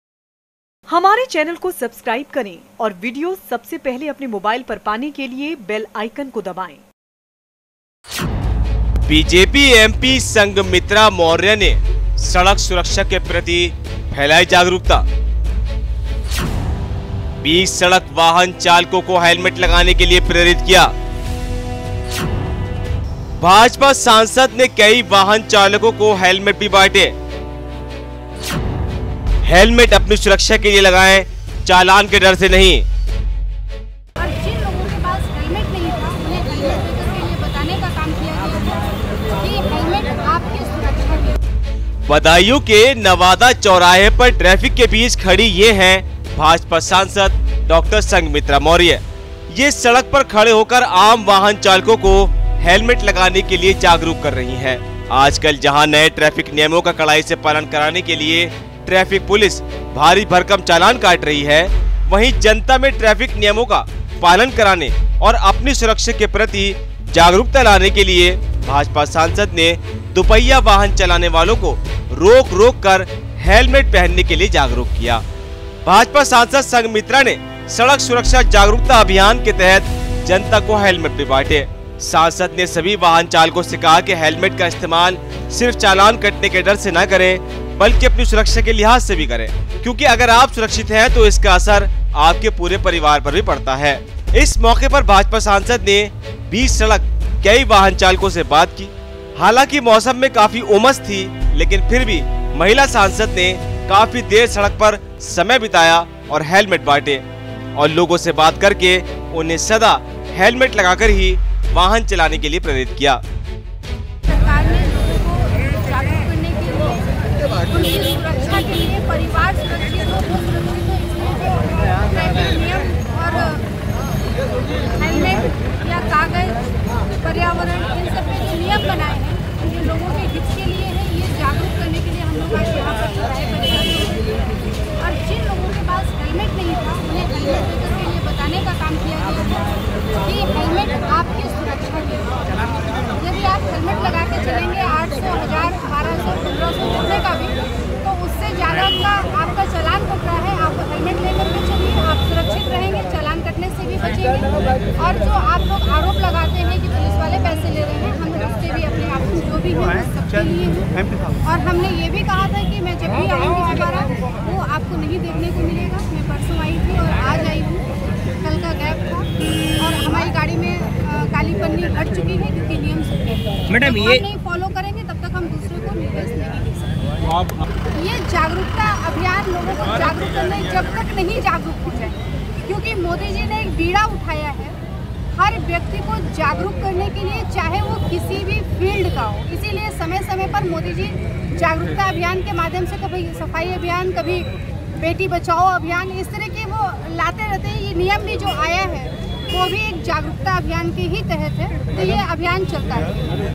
जब सड़क पर हेलमेट बाँटने लगी BJP MP संघमित्रा मौर्य !
BJP MP संघमित्रा मौर्य ने सड़क सुरक्षा के प्रति फैलाई जागरूकता,बांटे हेलमेट ।